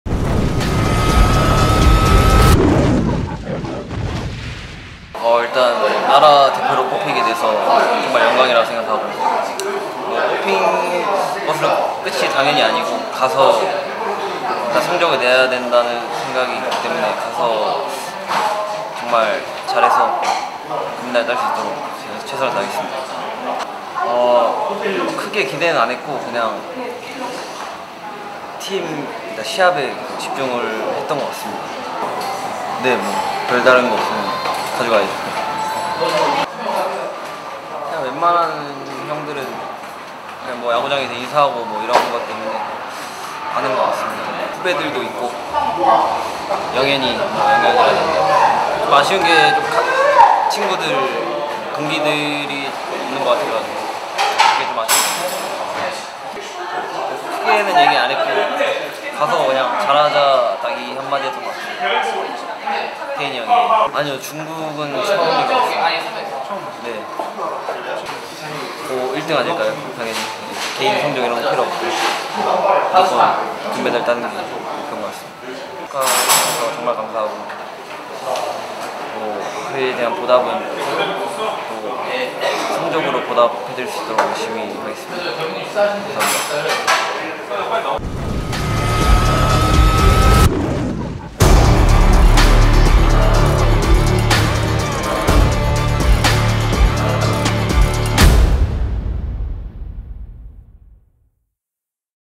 2022 항저우 아시안게임 발탁! 국가대표 김지찬 🇰🇷
잠실에서 만난 지찬선수! 국가대표 발탁 소감을 물어봤습니다
일단 태극기 좀 펄럭일게요 🇰🇷